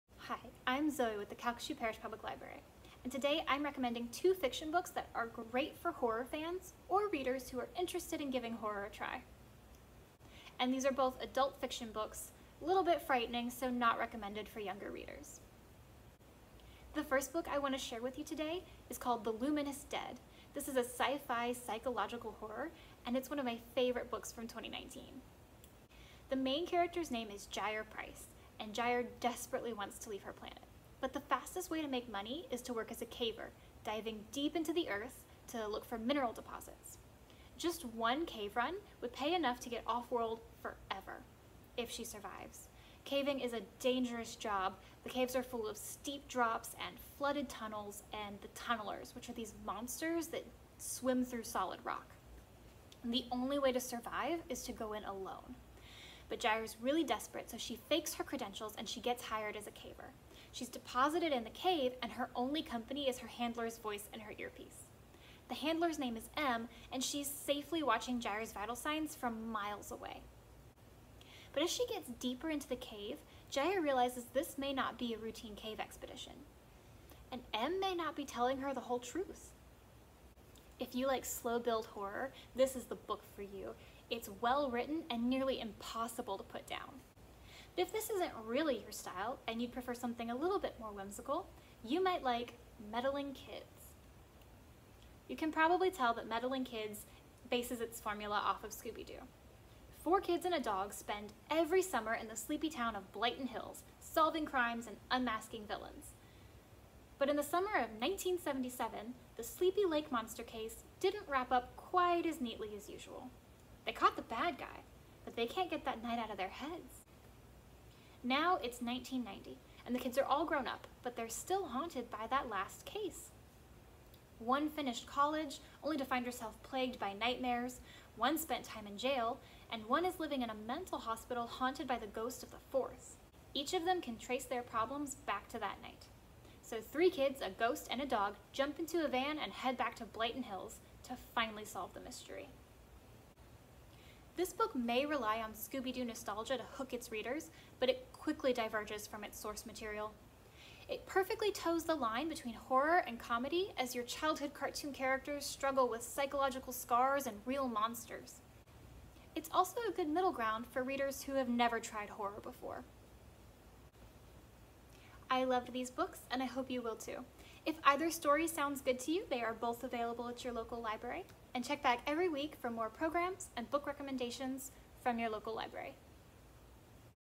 Booktalk: Adult Fiction for Horror Fans
This Adult Booktalk recommends two excellent fiction books for sci-fi and horror lovers. Books Recommended: "The Luminous Dead," by Caitlin Starling and "Meddling Kids," by Edgar Cantero.